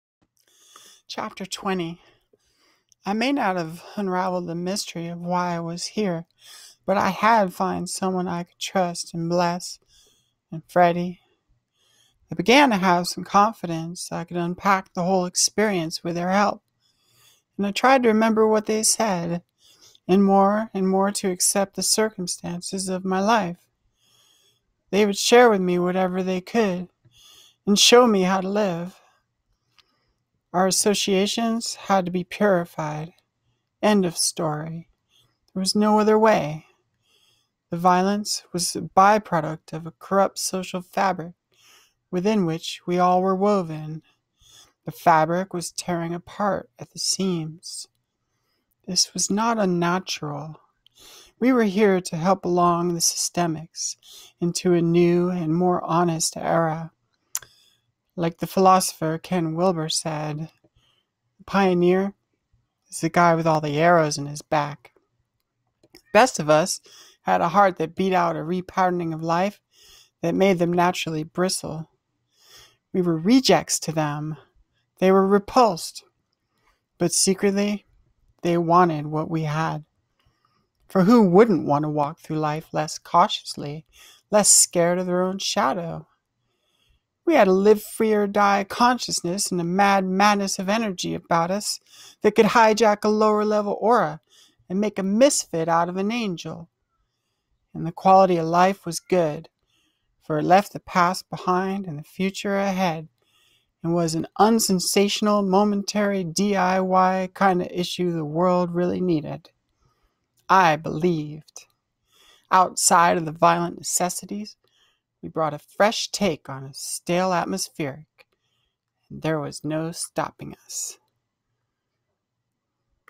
GTL BOOK 1.20.1
Created, written and read by Katya Mills from her novella 'Grand Theft Life'